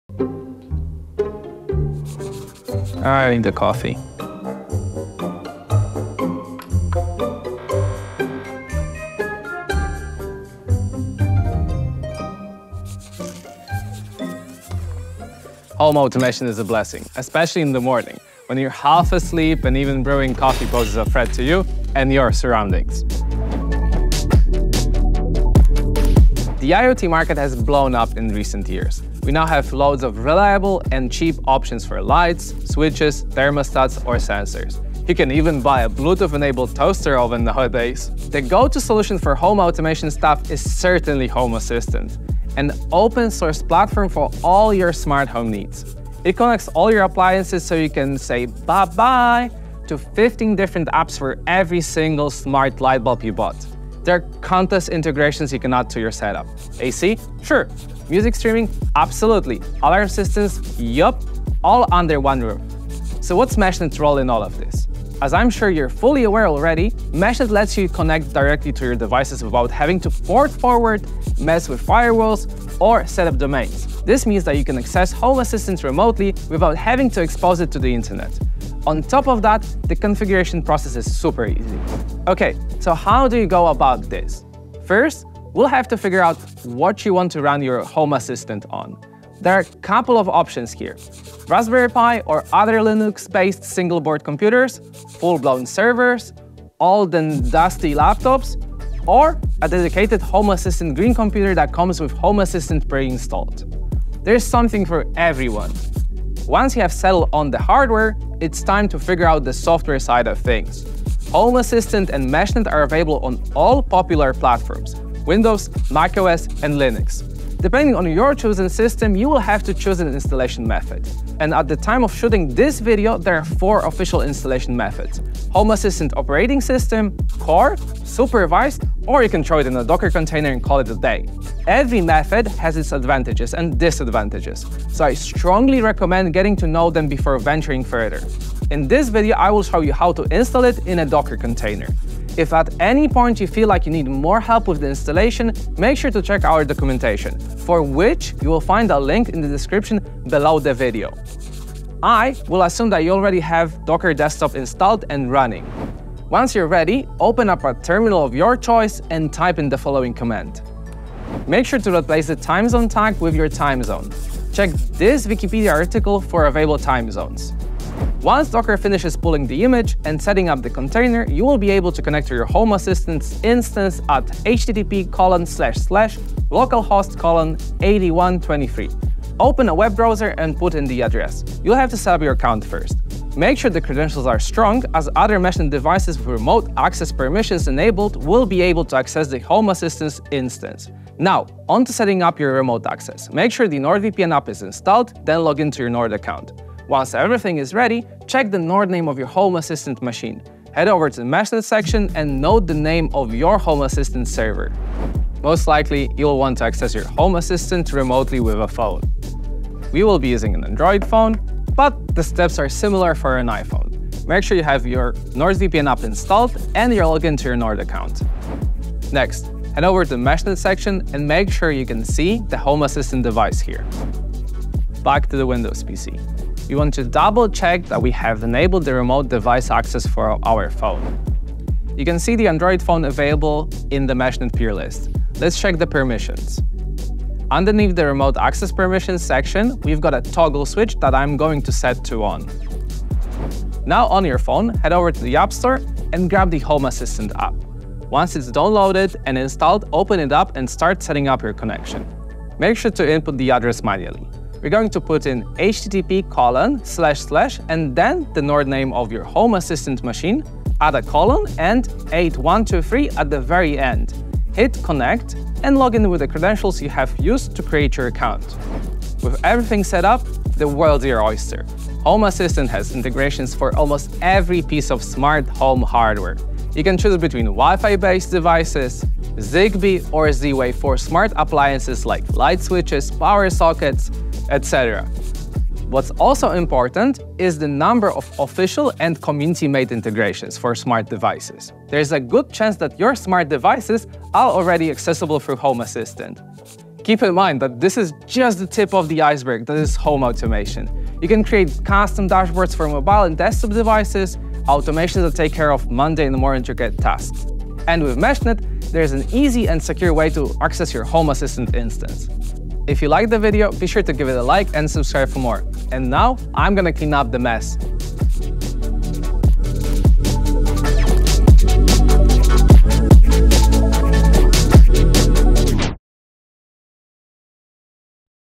How to set up Home Assistant with Meshnet | Easy tutorial
Access your devices remotely with Meshnet 📲 💻 🖥️

Home automation can be super convenient, but if you have to manage dozens of apps for each IoT device in your smart home, it can become a chore. This is why Home Assistant is a lifesaver — you can connect all your appliances in one platform. And if you use Meshnet, you can access Home Assistant remotely without having to expose it to the internet. On top of that, the configuration process is super easy.

Contents:

00:00 Intro
00:27 Before installing Home Assistant
02:31 How to install Home Assistant in a Docker Container
03:30 How to set up remote access with Meshnet
05:13 Outro

Before installing Home Assistant:
Figure out what you want to run Home Assistant on. There are a couple of options here: Raspberry Pi or other Linux-based single-board computers, full-blown servers, old and dusty laptops, or a dedicated Home Assistant Green computer that comes with Home Assistant preinstalled.

Once you have settled on the hardware, it’s time to figure out the software side of things. Home Assistant and Meshnet are available on all popular platforms – Windows, MacOS, and Linux. Depending on your chosen system, you will have to choose an installation method. There are four official installation methods:  Home Assistant Operating System, Core, Supervised, or you can throw it in a Docker Container and call it a day

In this video, I will show you how to install it in a Docker container.

How to install Home Assistant in a Docker Container:

1. Have Docker Desktop installed and running.
2. Open your terminal of choice and type in the following command:
docker run -d --name homeassistant --privileged --restart=unless-stopped -p 8123:8123 -e TZ=TimeZone -v homeassistant_config:/config
3. Make sure to replace the "TimeZone" tag with your time zone.
4. Check this Wikipedia article for all available time zones
5. Once Docker finishes pulling the image and setting up the container, you will be able to connect to your Home Assistant instance
6. Open a web browser and put in the address.
7. You will have to set up your account first. Make sure the credentials are strong because other Meshnet devices with remote access permission enabled will be able to access the Home Assistant instance.

How to set up remote access with Meshnet:

1. Open the NordVPN app.
2. Check the Nord name of your Home Assistant machine. Head over to the Meshnet section and note the name of your Home Assistant server.
3. On your Android phone, make sure you have installed the NordVPN app and you’re logged into your Nord Account.
4. Head over to the Meshnet section and make sure we can see the Home Assistant device here.
Go back to the Windows PC. Double-check that you have enabled remote device access for your phone.
5. You can see the Android Phone available in the Meshnet peer list, so check the permissions. 6. Underneath the “Remote access permissions” section, toggle the switch on.
7. On your phone, head over to the app store and grab the Home Assistant app.
8. Once it’s downloaded and installed, open it up and start setting up your connection. Make sure to input the address manually.
9. Put in “ and then the Nord name of your Home Assistant machine, add a colon and “8123” at the very end.
10. Hit “Connect” and log in with the credentials you have used to create your account.
11. With everything set up, the world’s your oyster. Home Assistant has integration for almost every piece of smart home hardware.